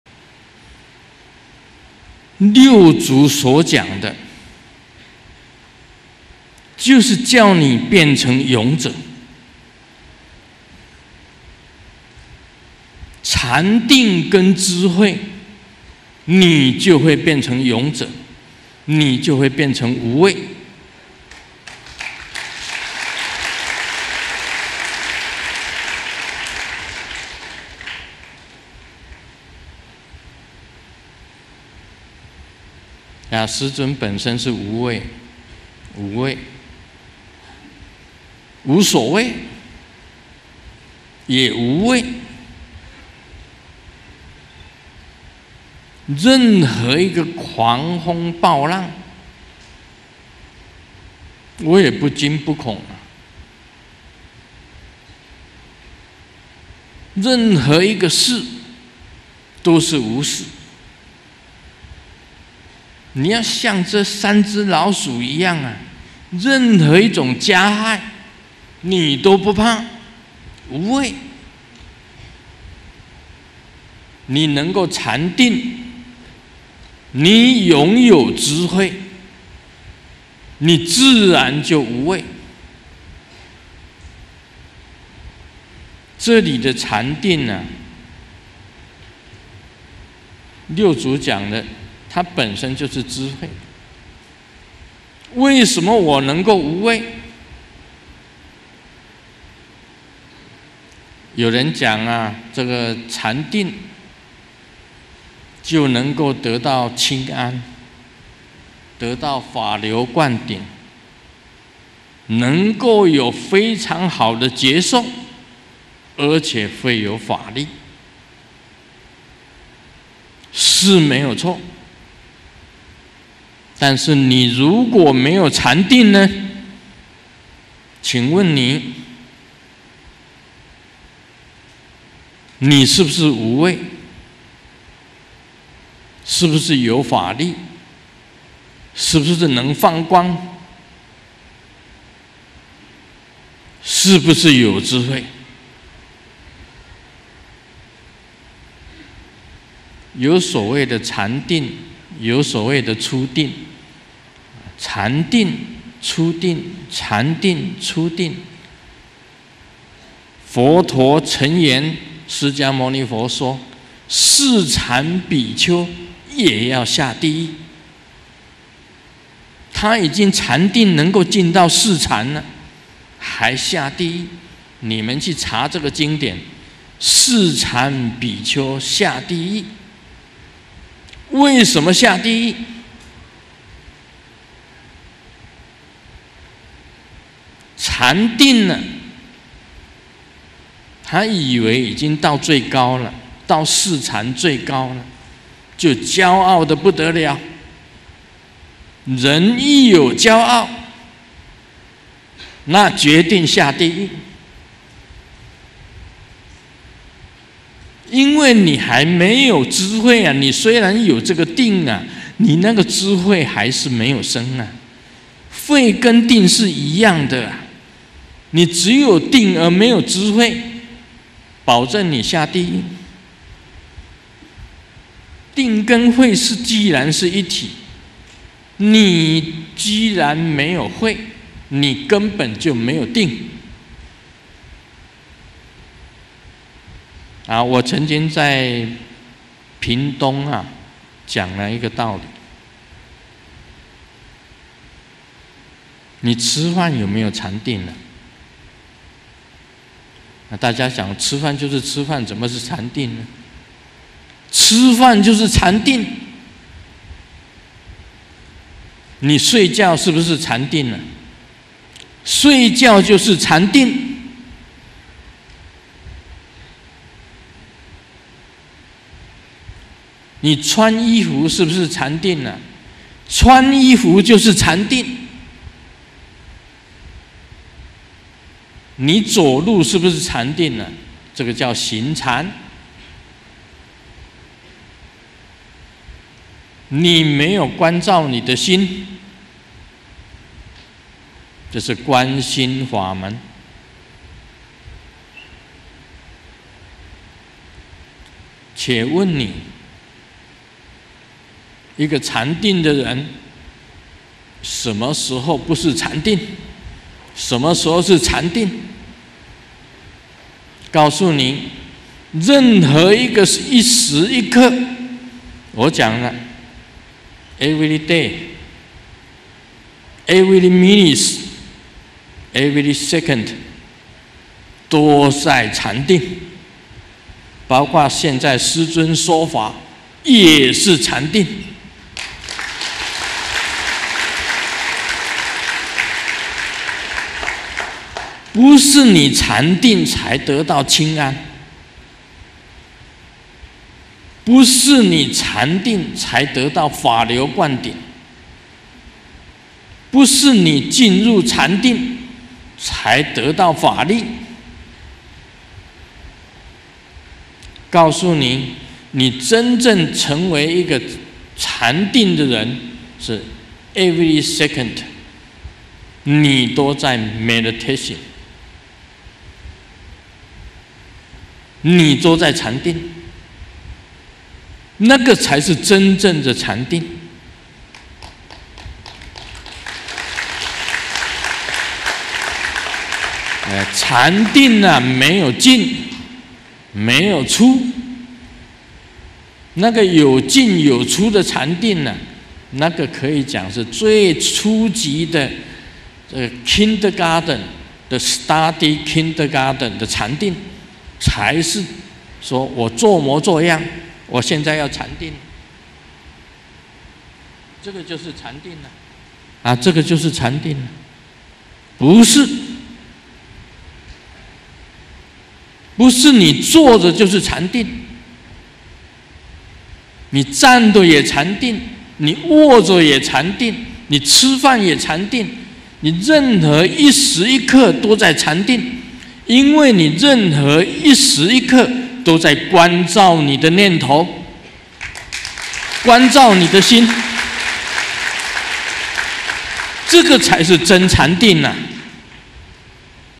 2010-07-17聖尊蓮生活佛盧勝彥法王主持「咕嚕咕咧佛母護摩大法會」暨講授密乘《六祖壇經》- 台灣雷藏寺中文直播 4/7
蓮生活佛盧勝彦講授 六祖壇經

法會主尊: 咕嚕咕咧佛母
灌頂:咕嚕咕咧佛母不共大法

定慧一體 無諍先後